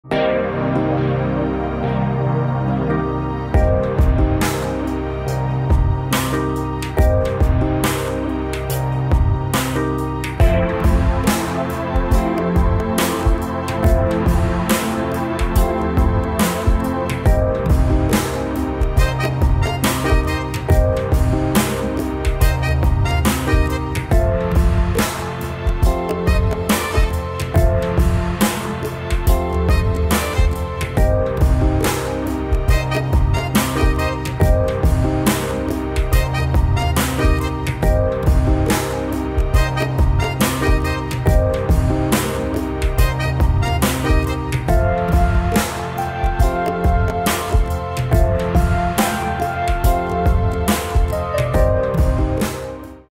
Slog3 Color Grade 4K (Sony a6500)
Sony a6500 w/ Vario-Tessar T* FE 16-35mm f/4 ZA OSS Lens
Picture Profile 8: Slog3, Cine3
LUT used: SGamut3CineSLog3_To_LC-709